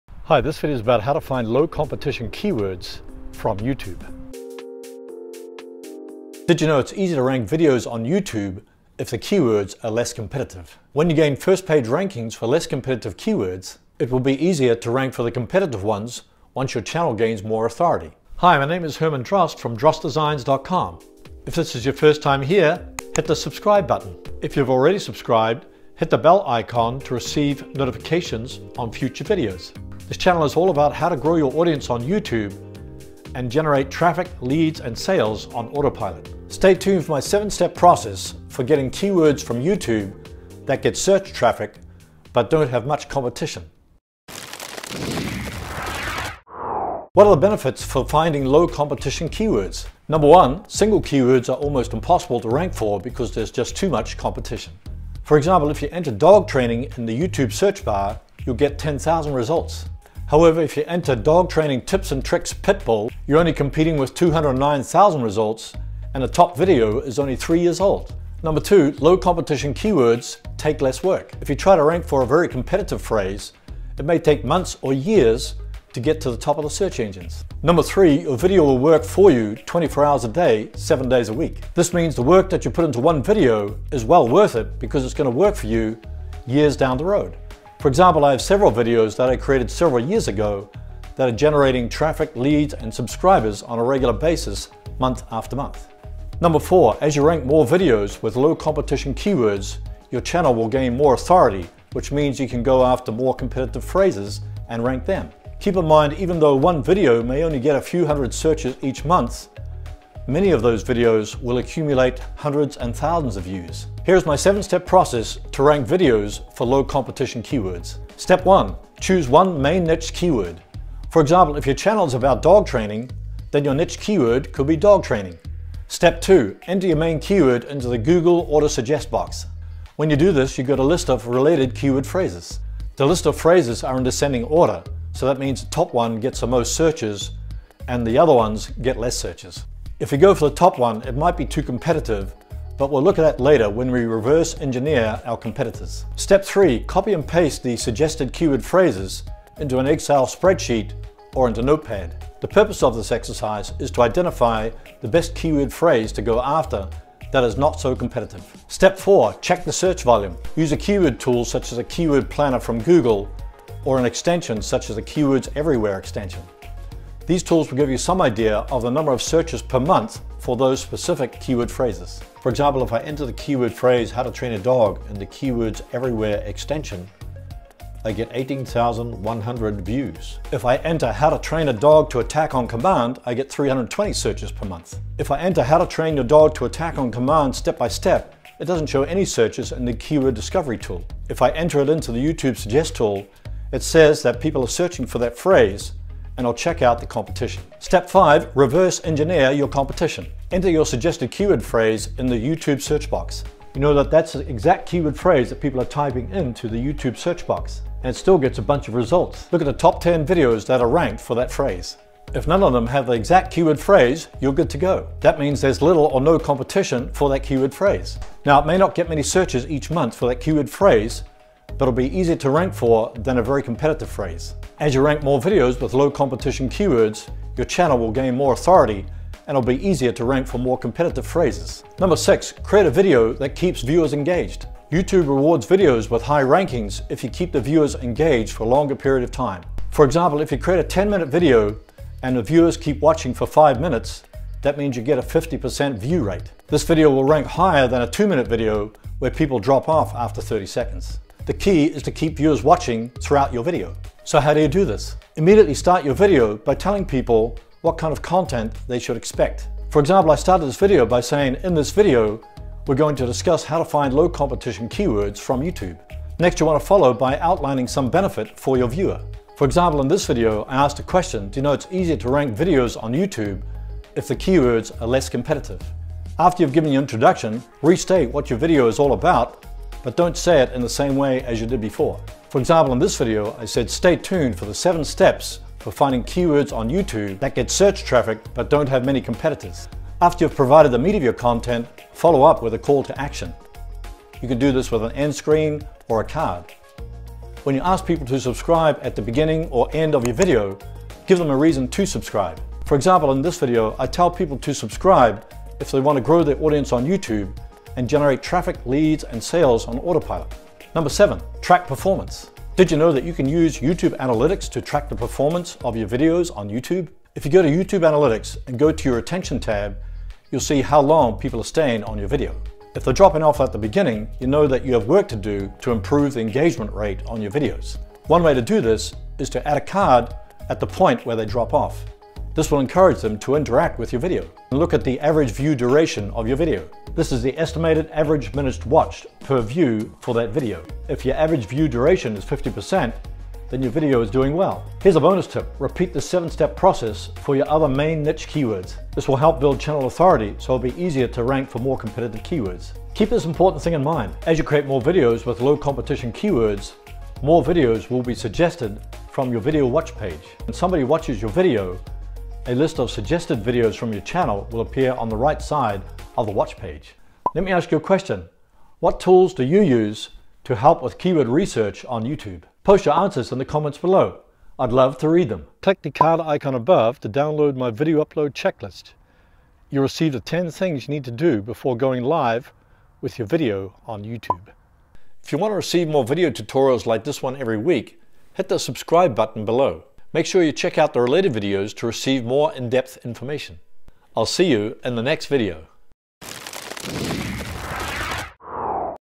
How To Find Low Competition Keywords From YouTube
Learn my 7 step process for How To Find Low Competition Keywords From YouTube. Did you know it’s easier to rank videos on YouTube if the keywords you choose are less competitive? When you gain first page rankings for less competitive keywords, it will be easier to rank for the competitive ones as your channel gains more authority.

Your channel will gain more authority as you upload more videos that generate search traffic from people discovering them on YouTube and/or Google.

Here is my 7 step process for finding  keywords from YouTube that get search traffic but don’t have many competitors...

Step 1.
Choose one main niche keyword
Step 2
Enter it into the YouTube Suggest Box
Step 3
Create a list of suggested keyword phrases
Step 4
Check the search volume
Step 5
Reverse engineer your competition
Step 6
Create a video that keeps viewers engaged
Step 7
Track video performance

Bonus Tip
Repeat this process for your other main niche keywords so you build channel authority. As your channel gains more authority it will be easier to rank for more competitive keywords.